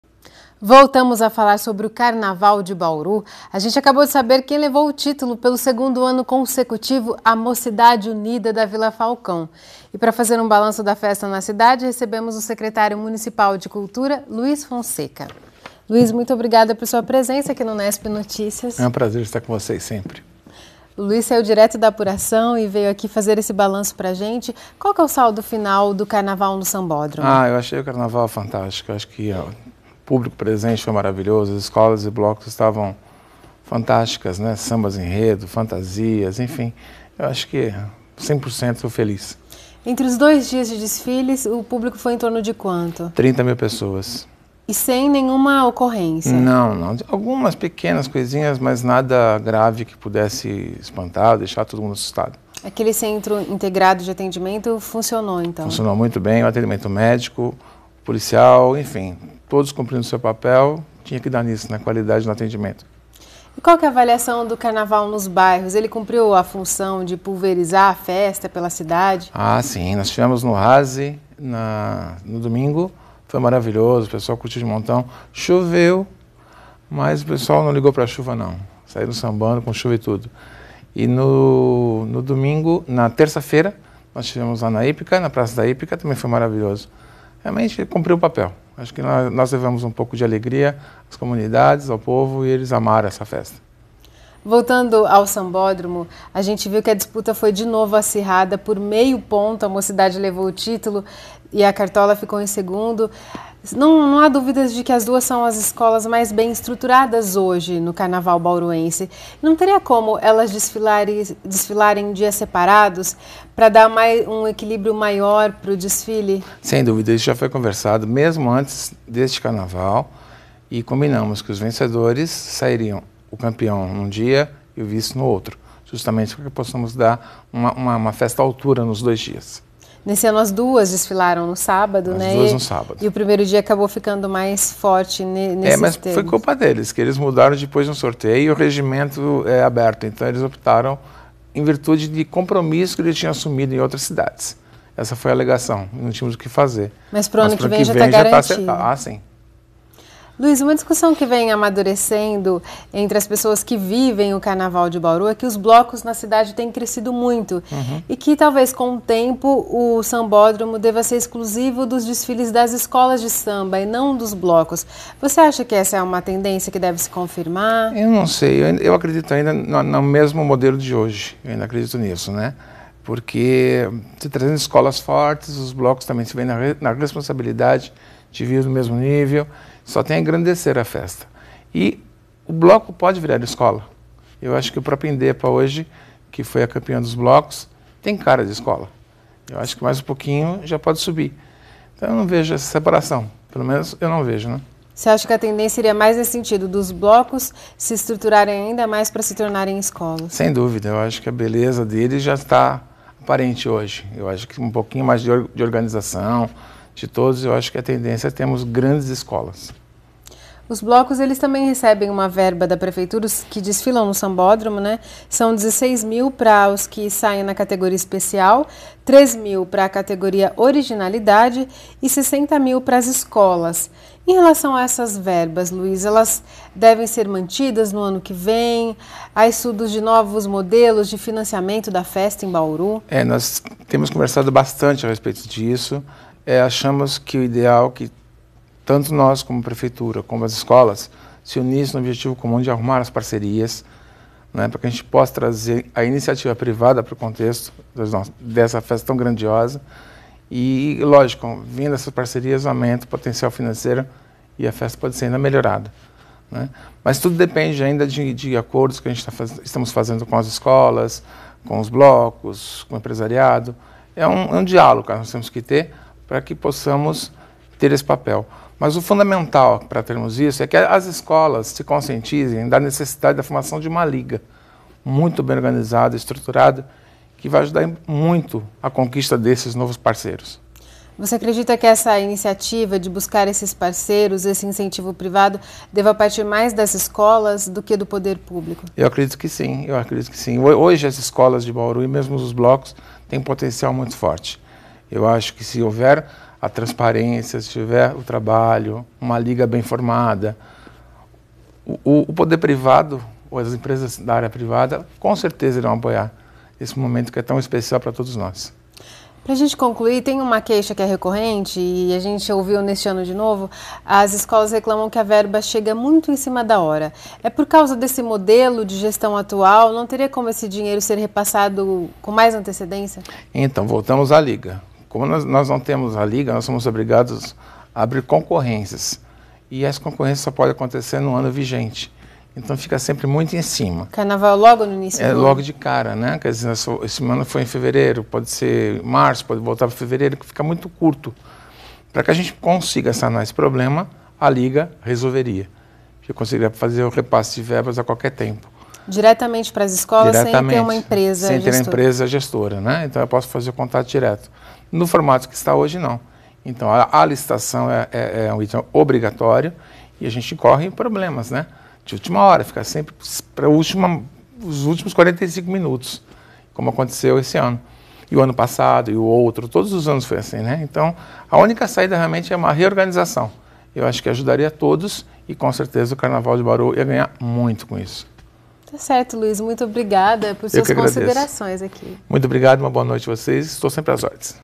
Unesp Notícias | Festa popular
Secretário de Cultura diz que liga de escolas de samba poderia melhorar a festa
Secretário de Cultura de Bauru, Luiz Fonseca, faz um balanço dos desfiles de Carnaval no Sambódromo. Para ele, problemas como a distribuição de verbas a apenas um mês do Carnaval podriam ser solucionados com a criação de uma liga que representasse as escolas de samba.